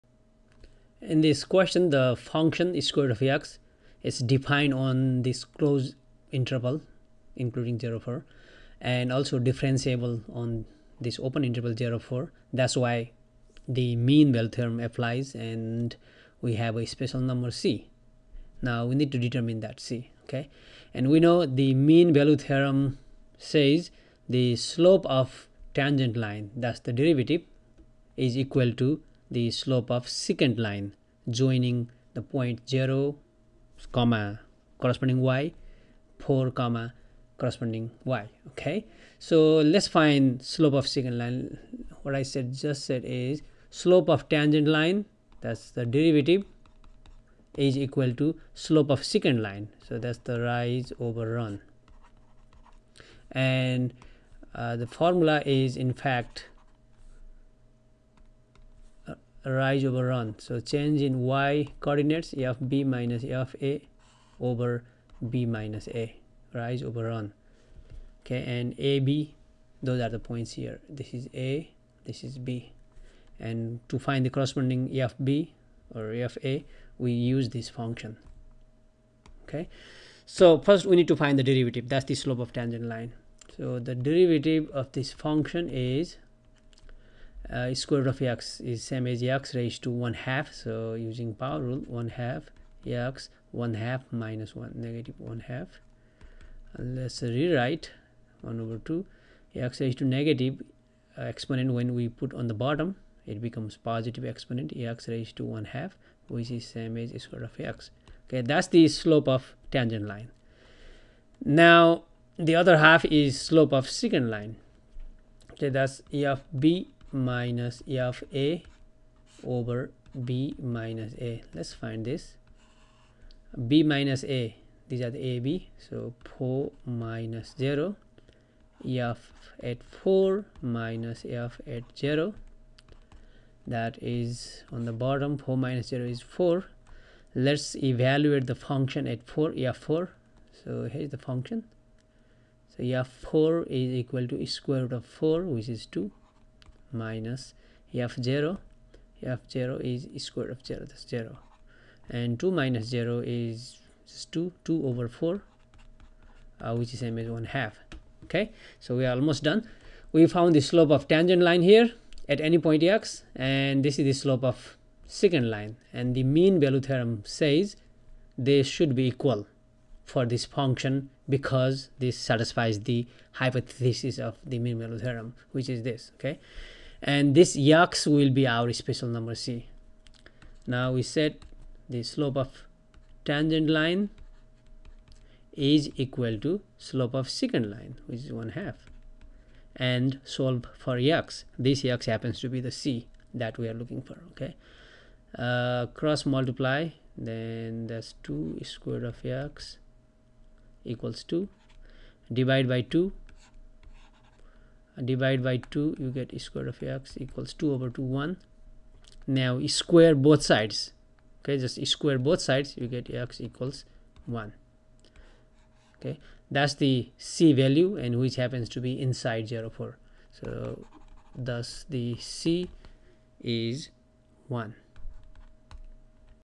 Finding c for f(x) = sqrt (x) in (0, 4) using The Mean Value Theorem
The Mean Value Theorem guarantees the existence of a special number c in the interval (0, 4)  for the function f(x)= sqrt(x) . Find the number c.